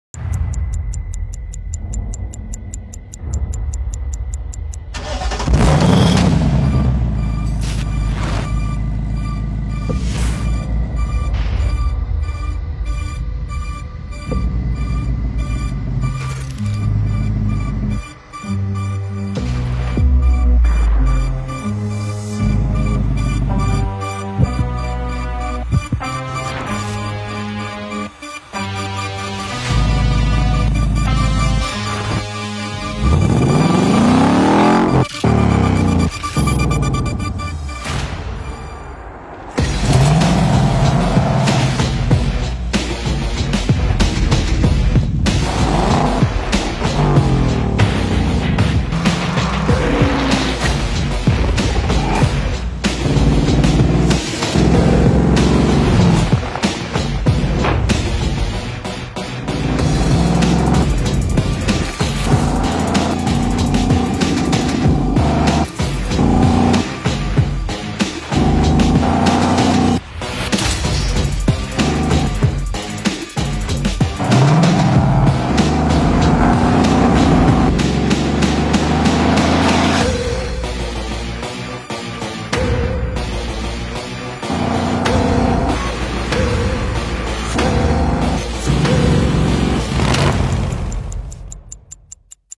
The all-new 2024 Ford Mustang is coming | Wayne Phillis Ford | Ford Dealer Adelaide
It’s time to wake up. The all-new 2024 Ford Mustang is coming.

Come along to Wayne Phillis FORD and experience the difference.